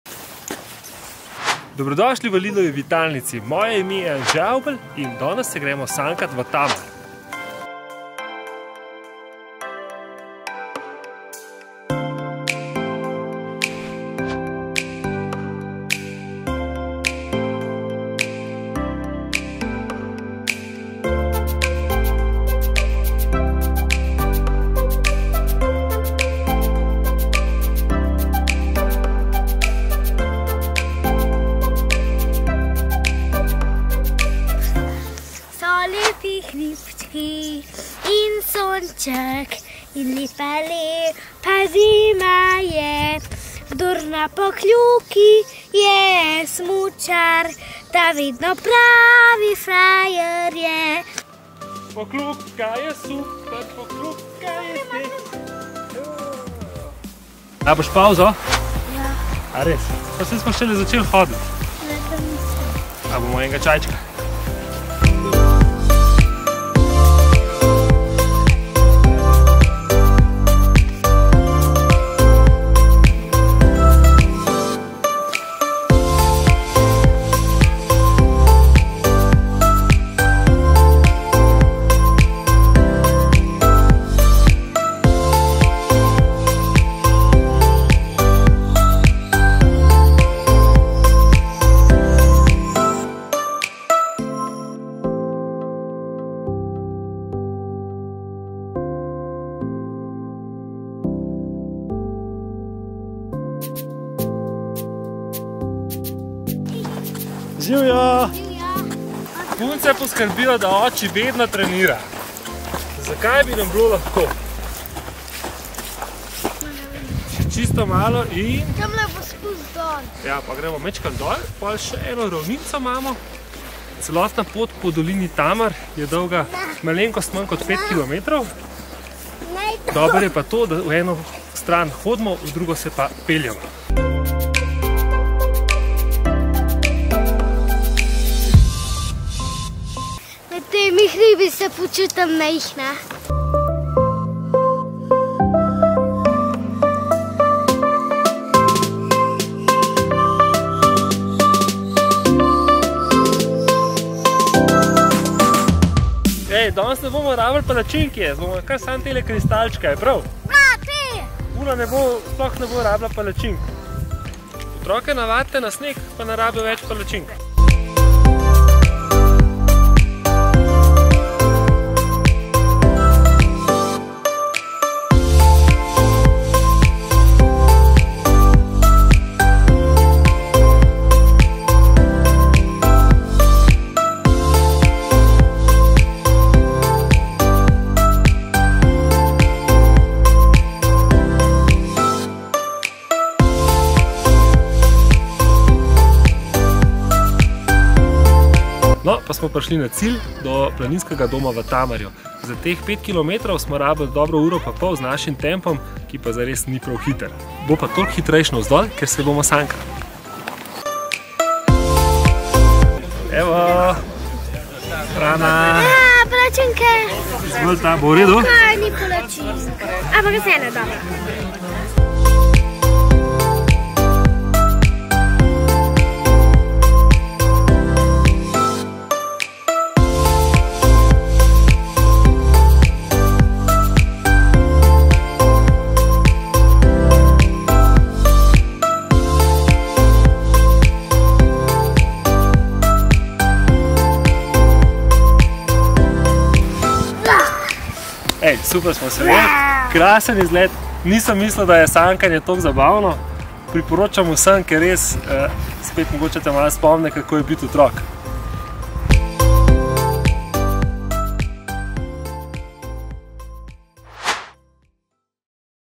Lidlova Vitalnica: Sankanje v pravljični dolini Tamar
Anže se je po uspešnem smučarskem podvigu s svojimi puncami, tokrat odpravil na sankanje v pravljično dolino Tamar. 🛷 Katero zimsko aktivnost bi pa ti raje izbral/a – smučanje ali sankanje? 👇

0:00 Uvod
0:20 odhod na sankanje
1:16 Opis poti
3:19 Planinski dom v Tamarju
3:48 Spust s sanmi
4:06 Zaključek
4:23 End screen